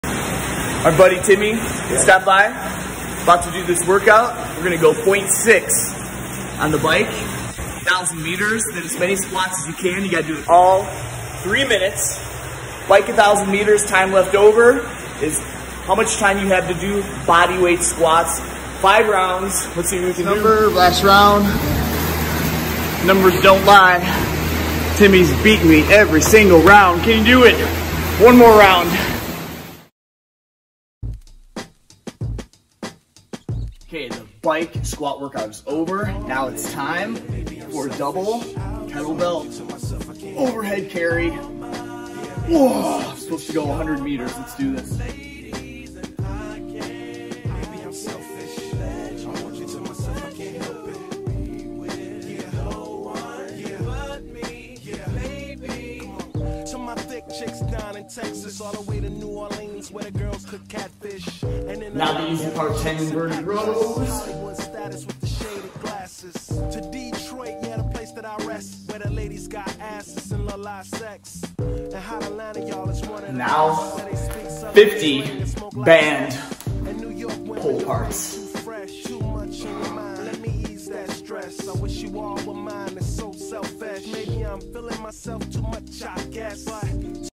Ultimate Leg Burning Workout from Crossfit
Check out this workout I originally got from The Buttery Brothers.

Set a clock for 3:00 mins.

Press start button and Bike 1000 meters, then as soon as you are don’t get off the bike and start doing Air Squats with the remaining time.

Rest 3:00 minutes then do it again.
5 Rounds total.

You get to rest for 3:00 minutes between rounds so this is a perfect Partner Workout.

The goal they told was 300 Squats total by the end of 5 rounds.

I’m only about 230 squats total currently.

I threw in some midline work I got from CompTrainafter I was done.
Do it and tell me what you think!!

Instagram.com/teamjamesoconnorofficial

My academy website
Team-Oconnor.com